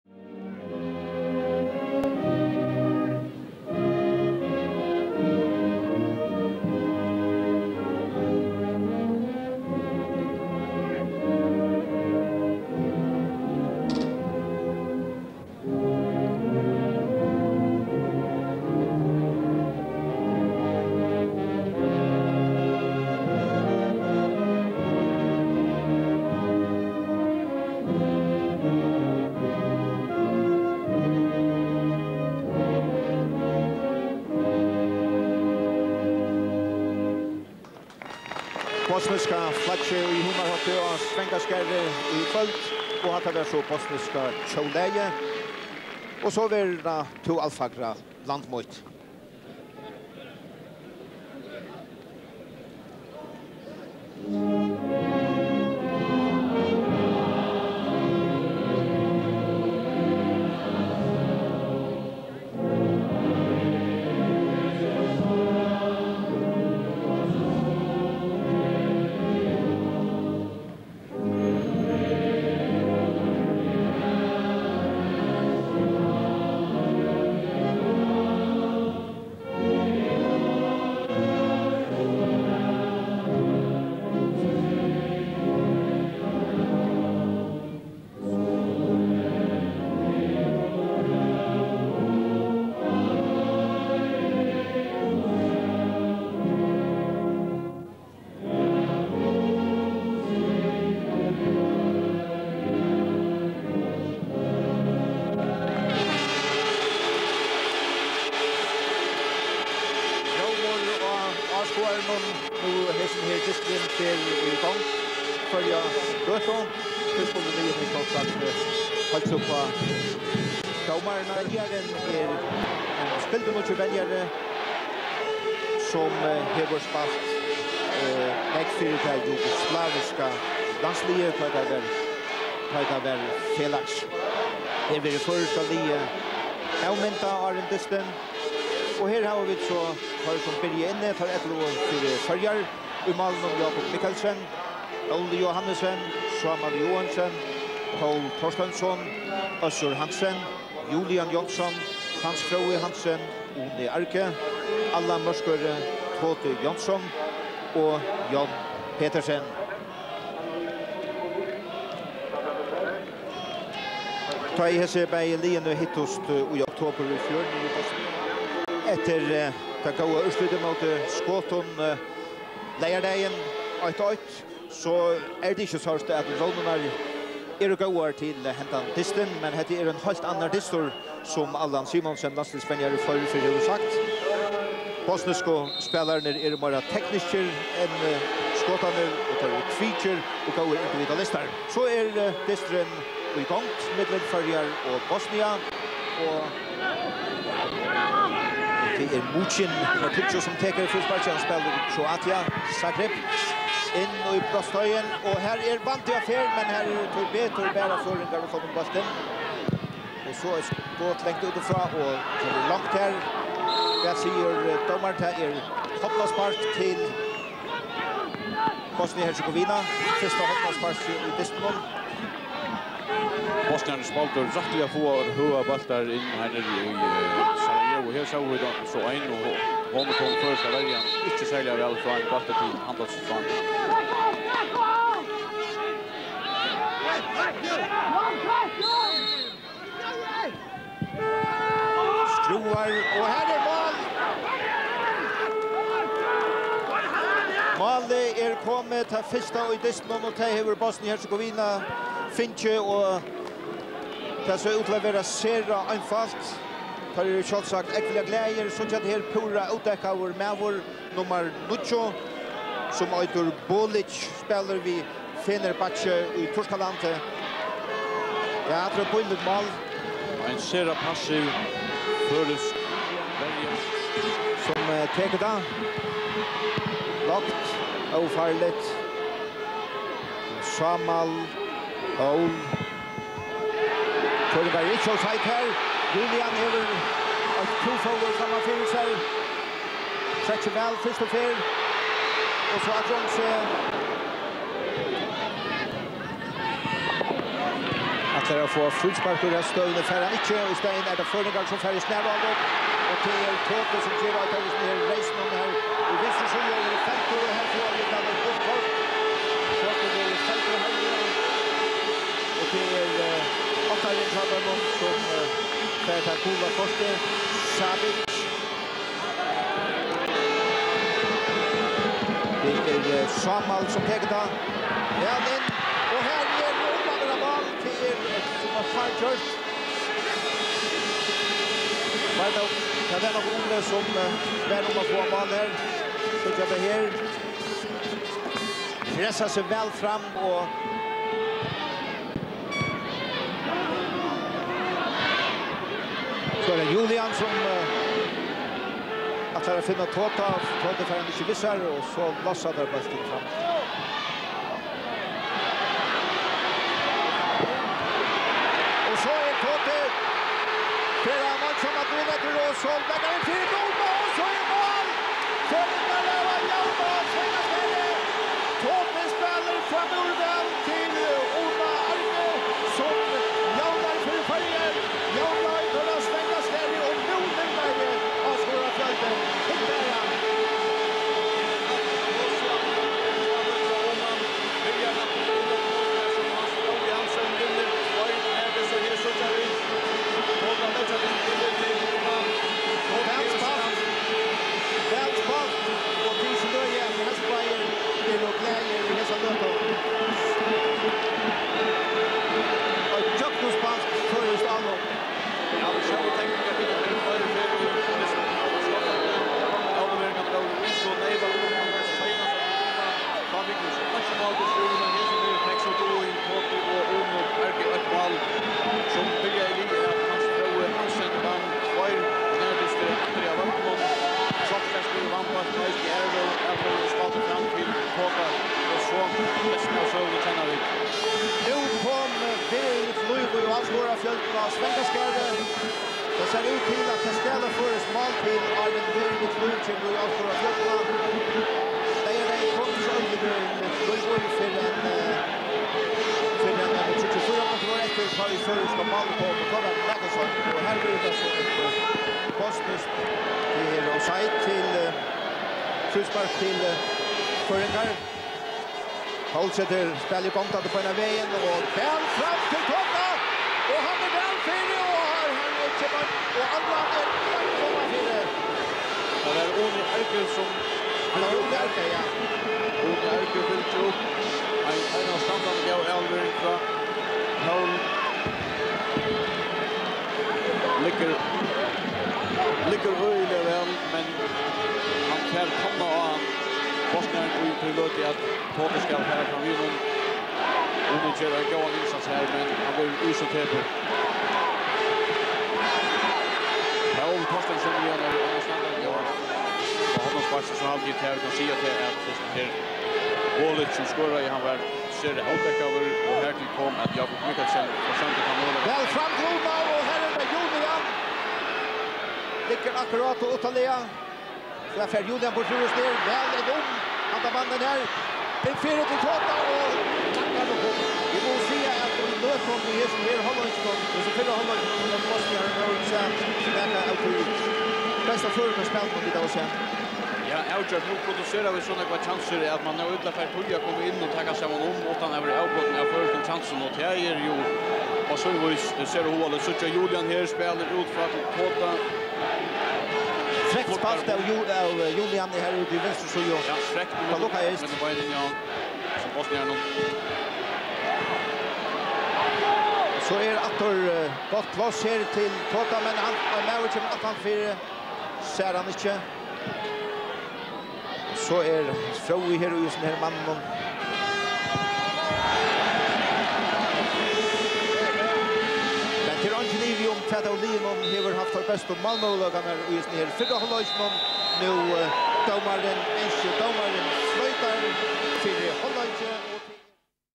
Faroes - Bosnia 2-2. Euro-2000 qualifiers. 1st half. Best ever Faroese display
This was one of the best ever performances by the Faroese National Team. More than 5000 spectators at fully packed rocky stadium enjoyed their team playing confident and at times classy football...

Euro-2000 qualifiers. Faroes 2 - Bosnia 2

June 9th, 1999. Svangaskarð stadium, Toftir, Faroes

Referee: Peter Jones (England).

0-1: Elvir Bolić 13
1-1: Uni Arge 38
2-1: Uni Arge 47
2-2: Elvir Bolić 49

Faroes:
1. Jákup Mikkelsen, Herfølge BK, Denmark
2. Óli Johannesen, TB Tvøroyri
3. Sámal Joensen, GÍ Gøta
4. Pól Thorsteinson, B36 Tórshavn
5. Øssur Hansen, B36 Tórshavn
(14. Henning Jarnskor, GÍ Gøta, 65)
6. Julian Johnsson, KIL Kongsvinger, Norway
7. Hans Fróði Hansen, HB Tórshavn
8. Uni Arge, Leiftur Ólafsfjörður, Iceland
(12. Jóhannis Joensen, HB Tórshavn, 87)
9. Allan Mørkøre, HB Tórshavn
10. Todi Jónsson, captain, FCK Copenhagen, Denmark
11. John Petersen, B36 Tórshavn
Coach: Allan Simonsen

Bosnia:
1. Mirsad Dedić, FK Sarajevo
2. Edin Smajić, NK Iskra Bugojno
3. Bakir Beserivić, FK Osijek, Croatia
4. Mirza Varešanović, Bursaspor Bursa, Turkey
5. Mirsad Hibić, FC Sevilla, Spain
6. Almir Turković, Zadarkomerc Zadar, Croatia
(14. Omer Joldić, FK Sloboda Tuzla, 63)
7. Senad Repuh, Bursaspor Bursa, Turkey
(17. Adnan Osmanhodžić, FK Sloboda Tuzla, 78)
8. Nermin Šabić, Kroatia Zagreb, Croatia
9. Elvir Bolić, captain, Fenerbahçe Istanbul, Turkey
10. Edin Mujčin, Kroatia Zagreb, Croatia
11. Marco Topić, Monza Calcio, Italy
(16. Samir Muratovič, Kocaelispor Izmir, Turkey, 87)
Coach: Faruk Hadžibegić